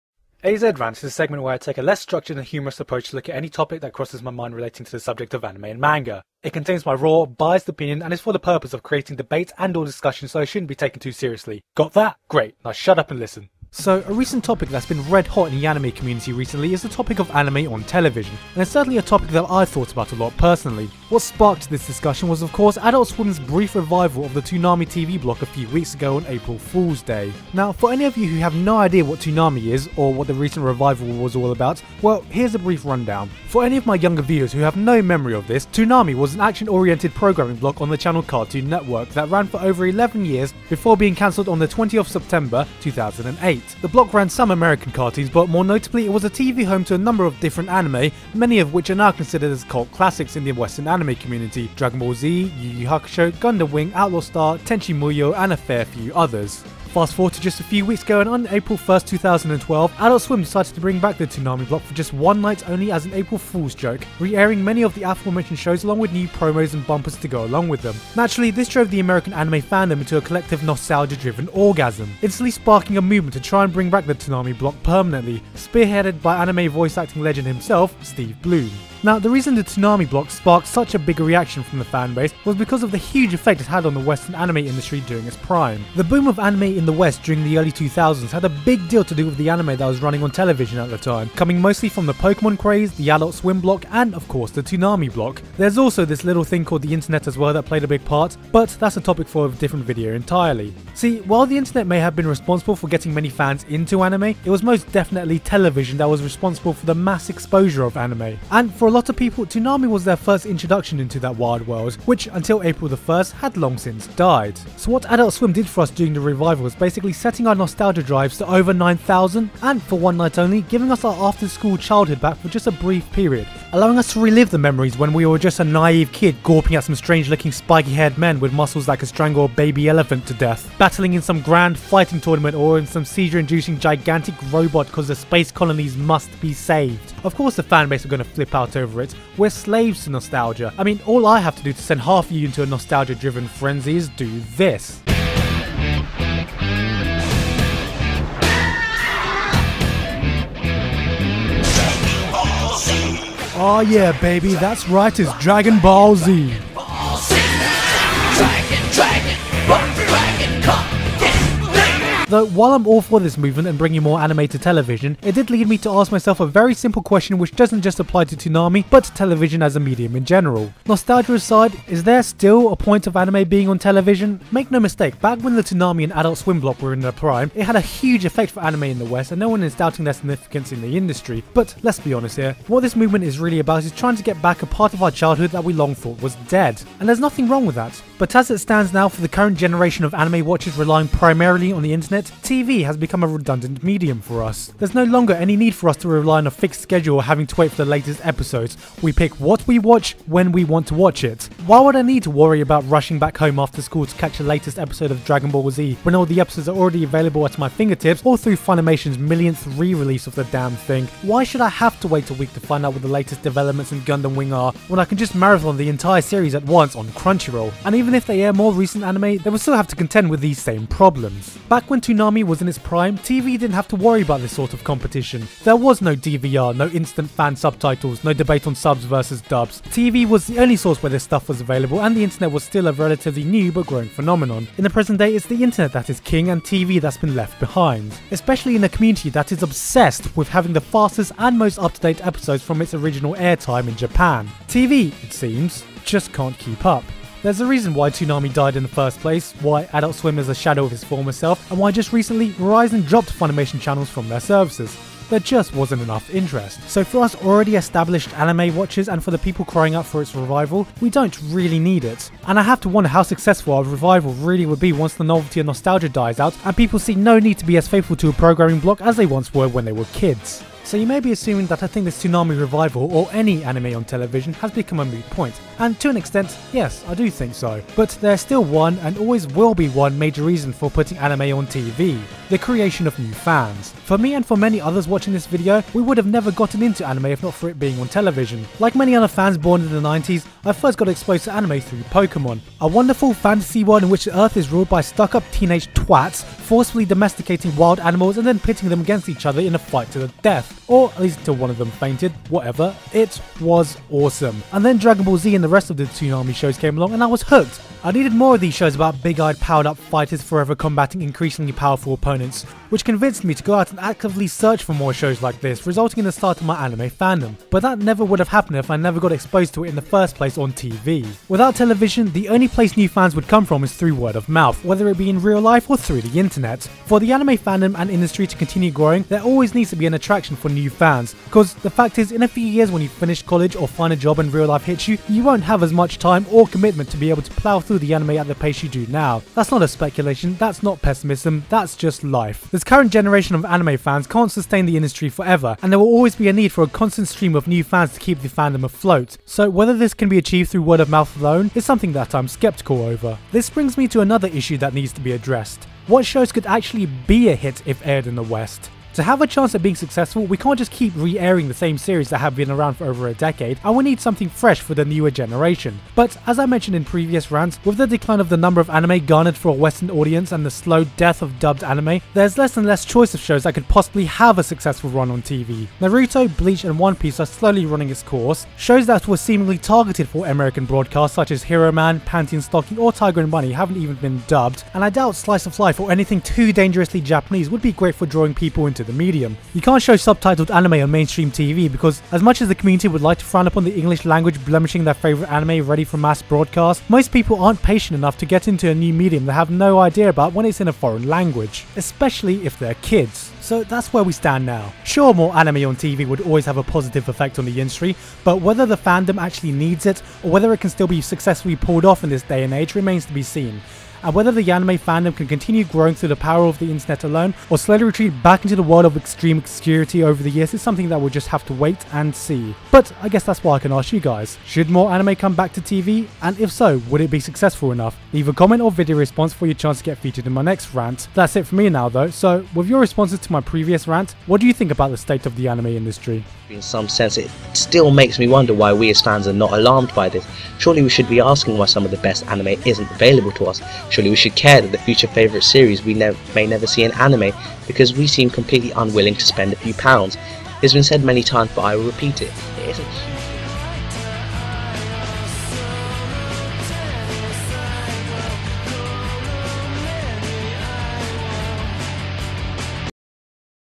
AZ Rant: Anime on TV
Thank you for OVER 9000! Subs. So lets celebrate with another anime rant. Today I talk about the recent discussion about anime on TV after the recent toonami revival by adult swim on April fools day.